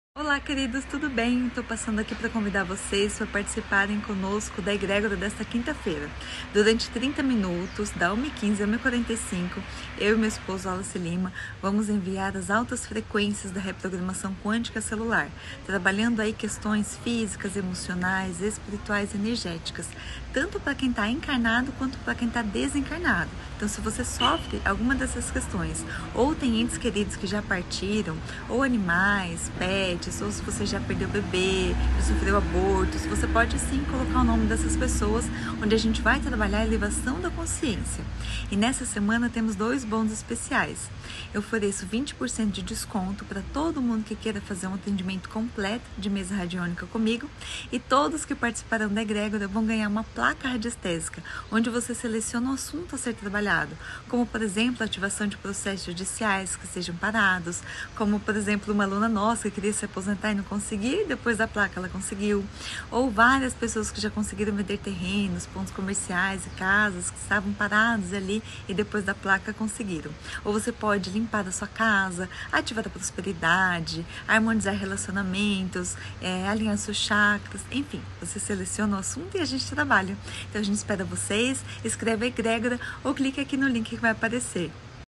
GANHE UM ATENDIMENTO COM PLACA RADIÔNICA AO PARTICIPAR DA EGRÉGORA | Wallace Lima & Evelline
Ganhe um ATENDIMENTO COM PLACA RADIÔNICA ao participar da Egrégora Quântica de Cura (EQC), amanhã, quinta, das 13:15 às 13:45.

Isso mesmo, ao participar da EGRÉGORA QUÂNTICA DE CURA (EQC) de amanhã, além de receber a poderosa vibração de cura da Reprogramação Quântica Celular (RQC), durante 30 minutos, você ganhará um atendimento com a Dra Evelline Carvalho, com a PLACA RADIÔNICA.

Escolha uma ÁREA da sua VIDA que está BLOQUEADA que Evelline irá usar Gráficos Radiestésicos específicos, de alta frequência, para mobilizar a energia necessária para DESBLOQUEAR sua vida numa área específica.

Além disso, você também poderá agendar um Atendimento de MESA RADIÔNICA com Evelline Carvalho para um olhar mais amplo sobre sua vida e ganhar um desconto de 20% no atendimento.

CONFIRA TODOS OS BÔNUS DA SEMANA👇

1. Um atendimento com Placa Radiônica com Dra Evelline Carvalho. Você escolhe um tema prioritário para trabalhar na sua vida e Evelline vai fazer seu atendimento, num prazo de até 30 dias, por ordem de inscrição. De R$ 245 e será de graça para você.

2. Desconto de 20% no atendimento com Mesa Radiônica com Evelline Carvalho. Economia de R$ 106 no atendimento

4. Três meses de Assinatura do Programa Despertar Quântico por R$ 1 ao mês: Receba um Vídeo diário para elevar sua vibração e elevar sua imunidade. *De R$ 150, por R$ 1 por mês. *

Clique no link abaixo para se inscrever na EGRÉGORA QC de amanhã e garantir seu atendimento com PLACA RADIÔNICA e demais bônus.👇